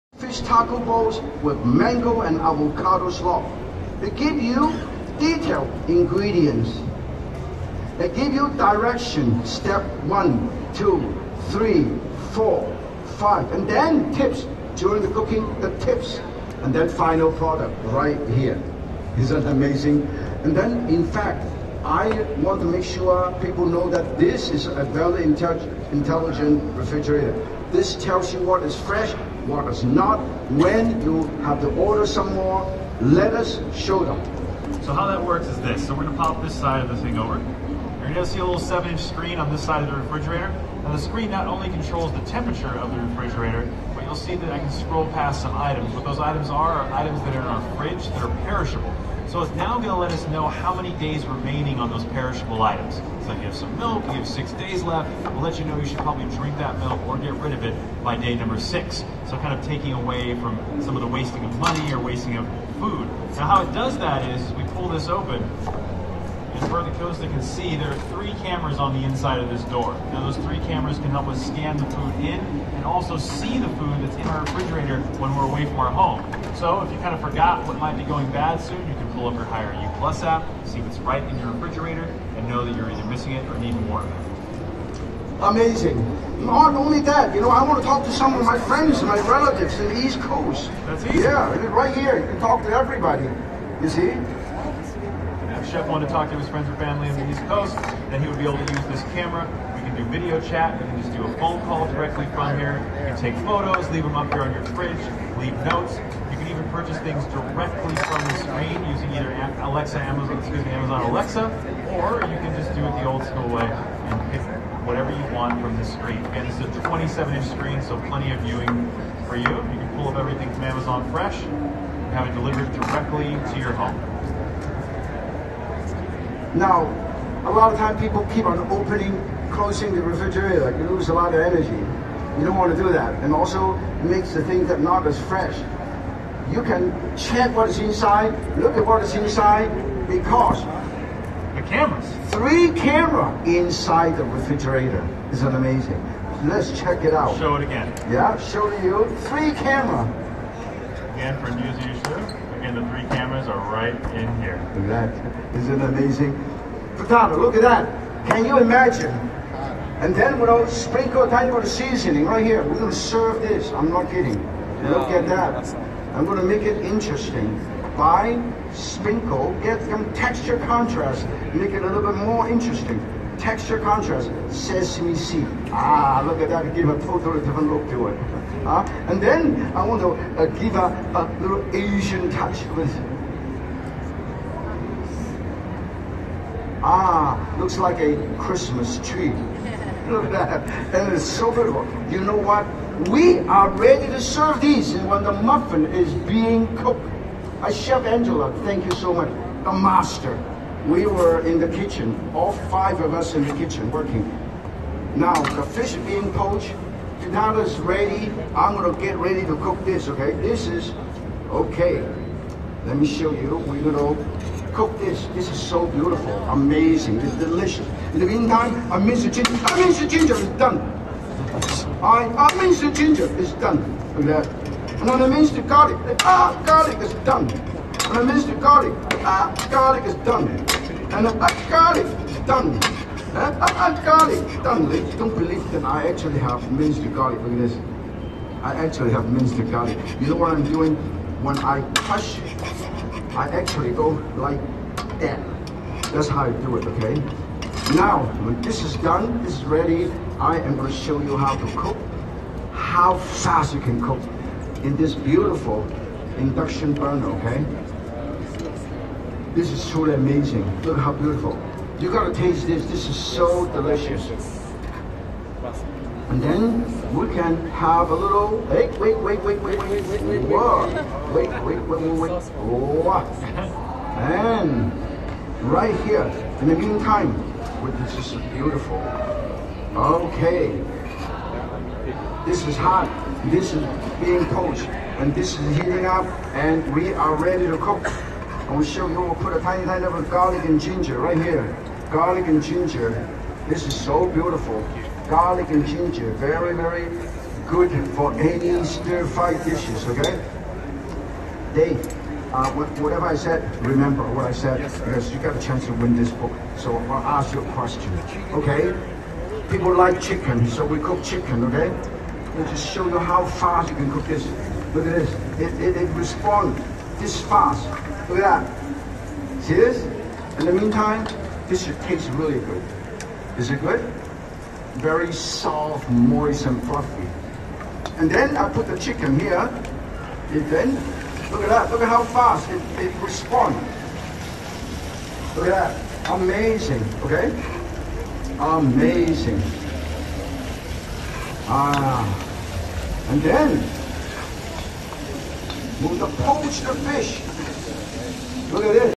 Yan Can Cook Smart Kitchen Presentation Part 2, CES 2020 [4K]
Hair - Smart Kitchen

Video taken at CES 2020 from Haier booth.

All content in this video is owned by GE Appliances, a Haier company.